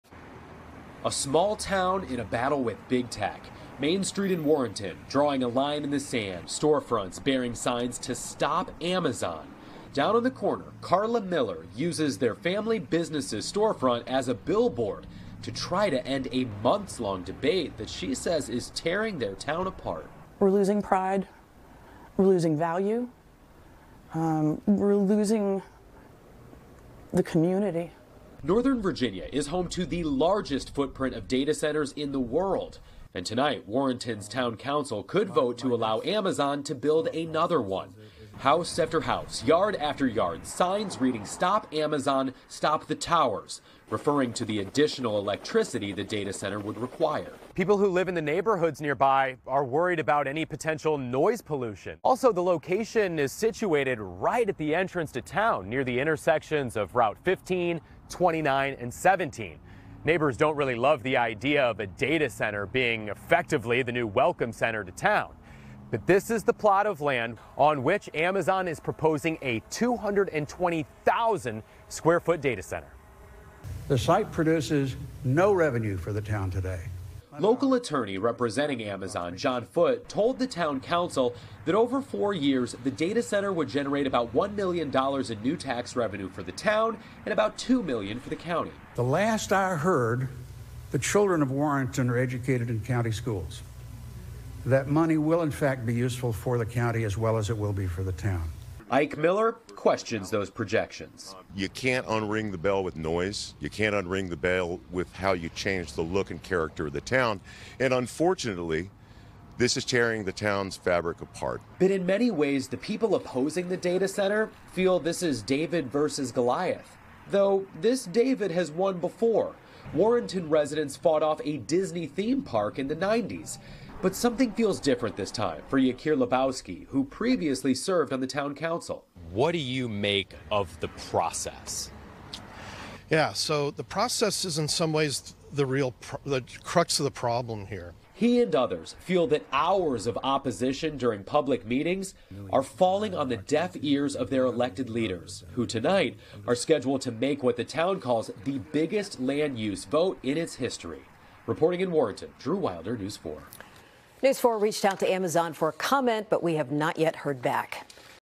Warrenton Residents Protest Amazon Data Center Proposal | NBC4 Washington
Virginia is home to the largest footprint of data centers in the world, and Warrenton’s town council could vote Tuesday night to allow Amazon to build another one. Drew Wilder reports.